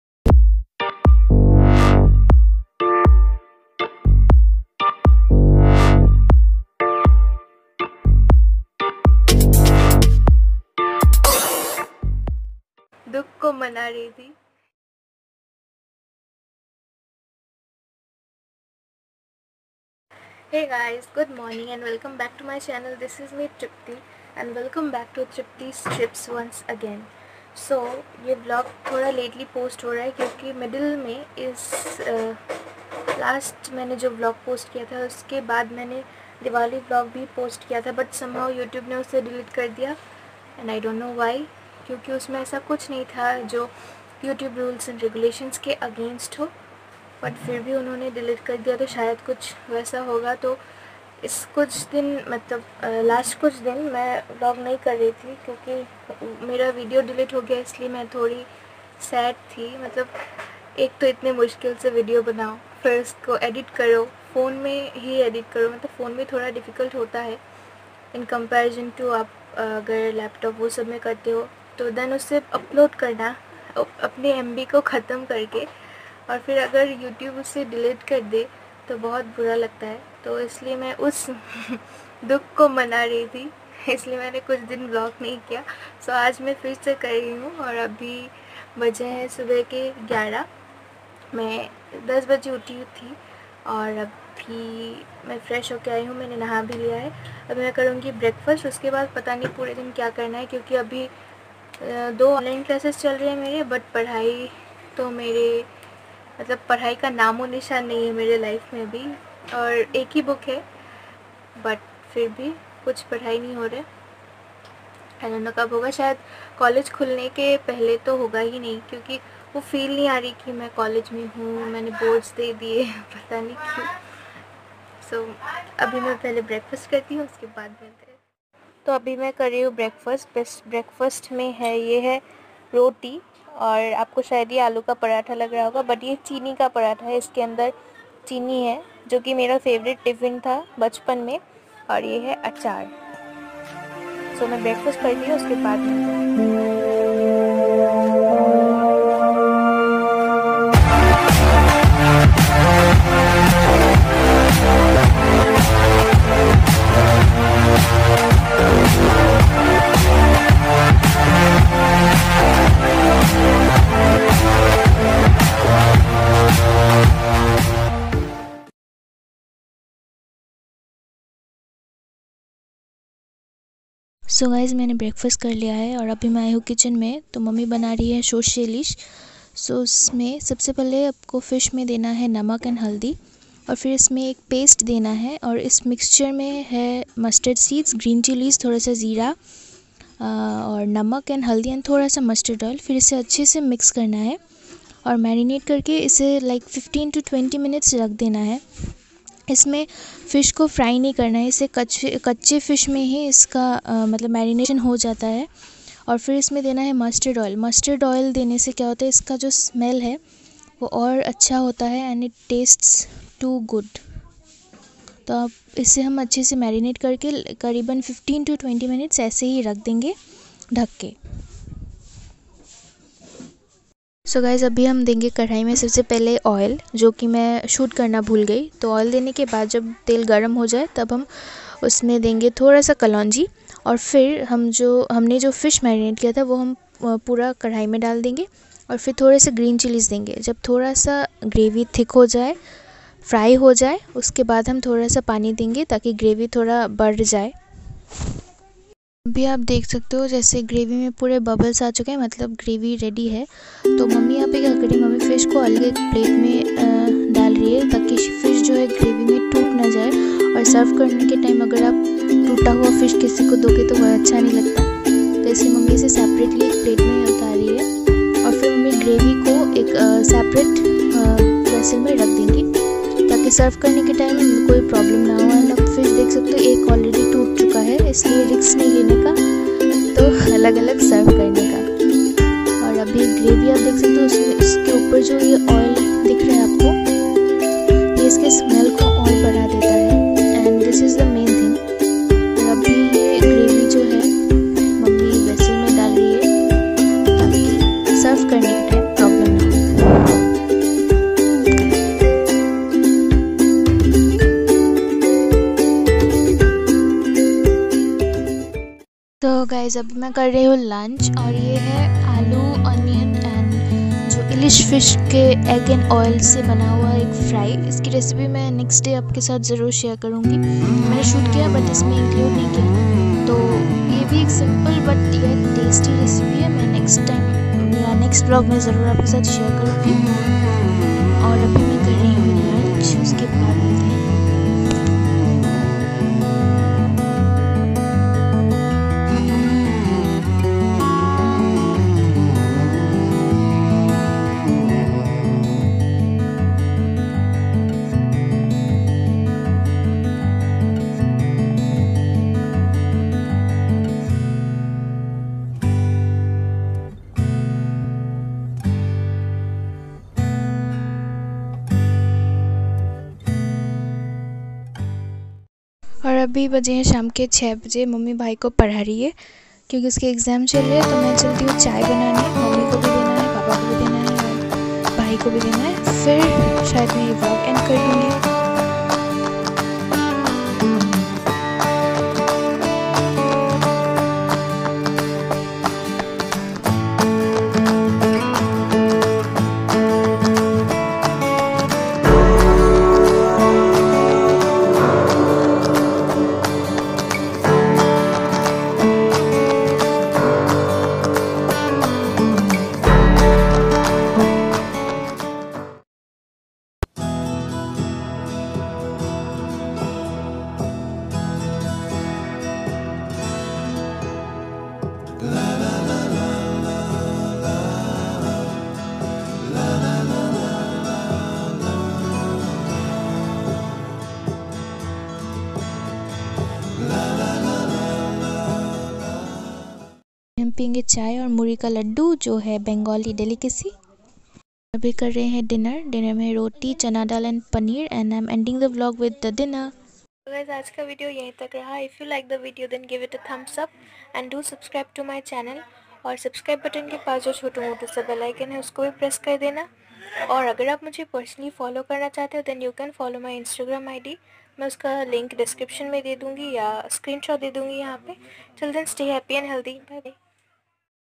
Vlog10❤️/SHORSHE ILISH EPISODE ❤️/Breakfast to dinner whole day❤️/Tripti's Trips ❤️trippyfam❤️
hey lovelies ❤️

stay happy and healthy ❤️
bye bye 👋😘